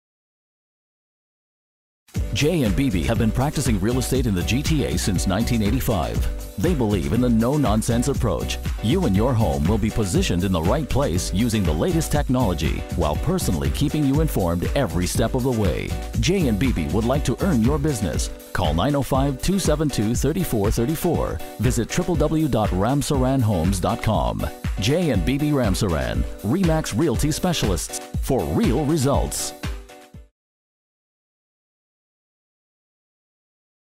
MEET JAY & BIBI RAMSARAN
MEET TWO OF THE GTA's HARDEST WORKING NAMES IN THE REAL ESTATE COMMUNITY... JAY & BIBI RAMSARAN FROM RE/MAX REALTY SPECIALISTS INC.  VISIT THEIR WEBSITE OR GIVE THEM A CALL AND GET JAY & BIBI WORKING FOR YOU.